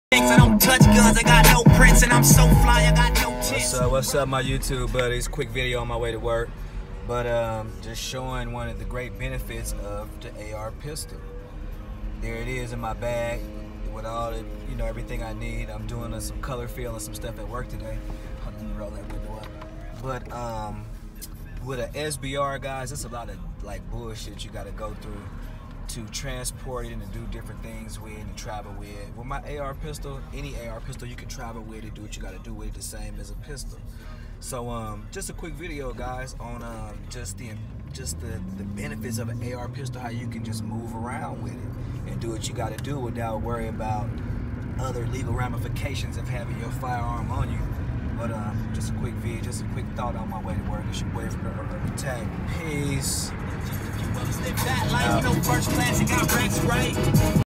AR pistol transportability
Just another quick video and quick thought on why the AR pistol is just as effective if not more beneficial than an SBR, just my opinion..